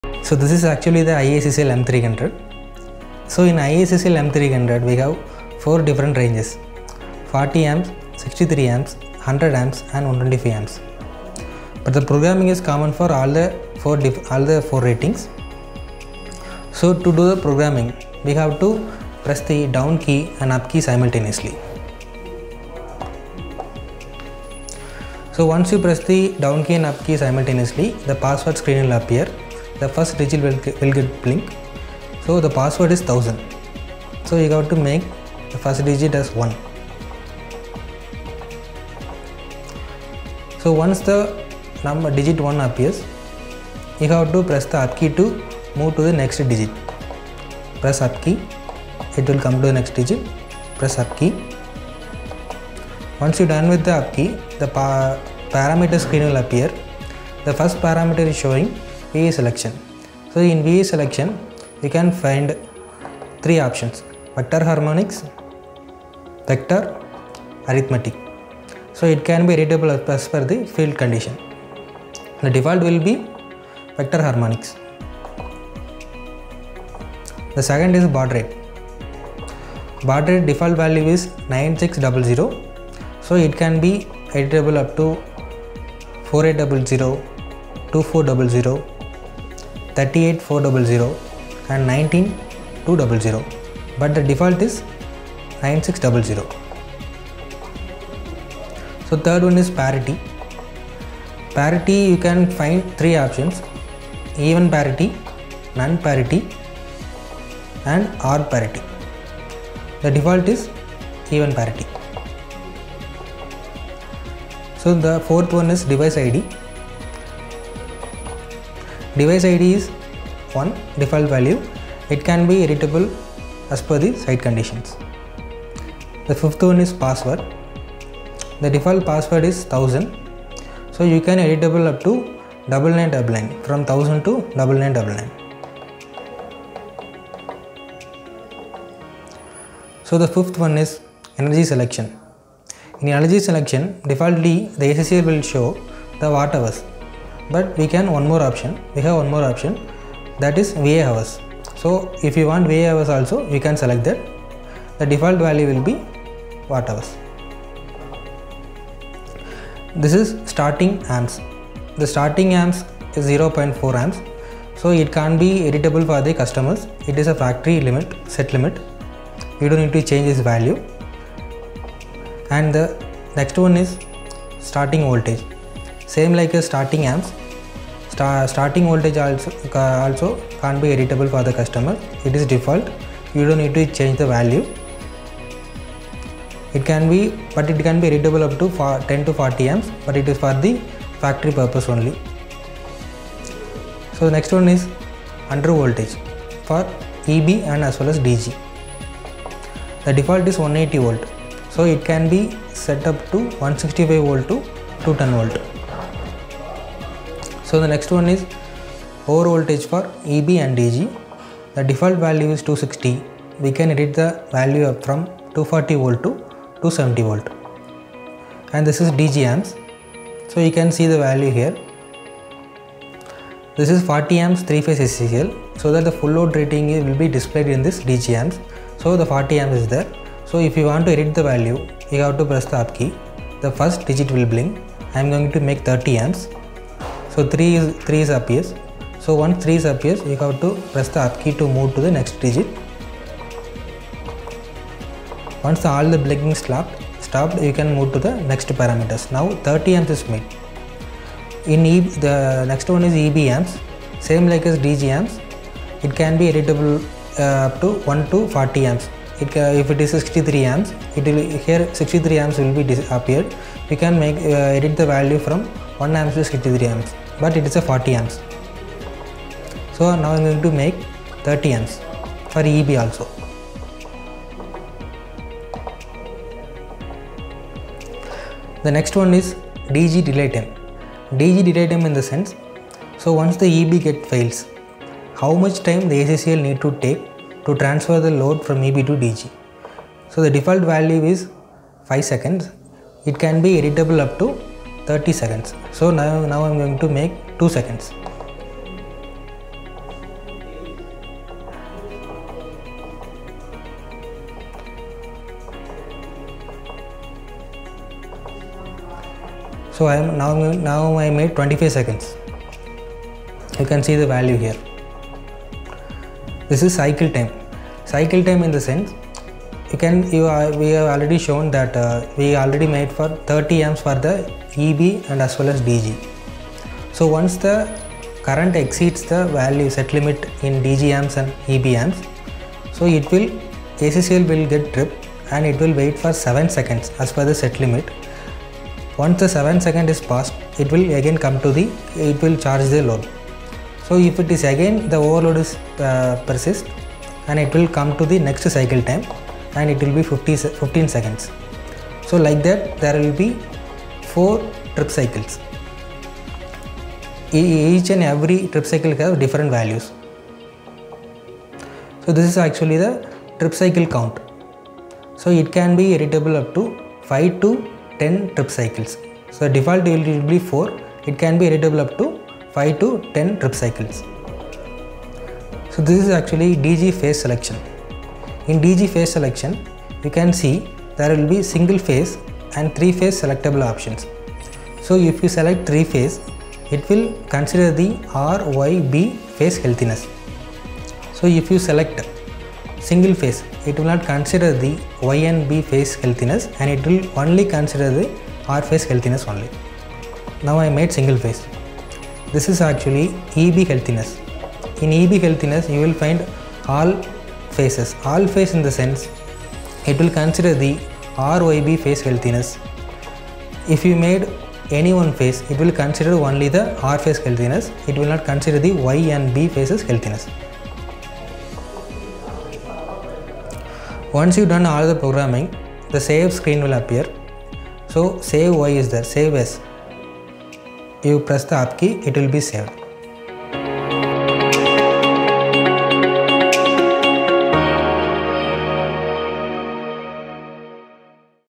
How to program ACCL M300 | Programming guide
In this video, we'll take you through a step-by-step guide on how to program the Elmeasure IACCL M300. By the end of this video, you'll have a comprehensive understanding of how to program the Elmeasure IACCL M300, enabling you to easily programme your automatic changeover with current limiter [ACCL].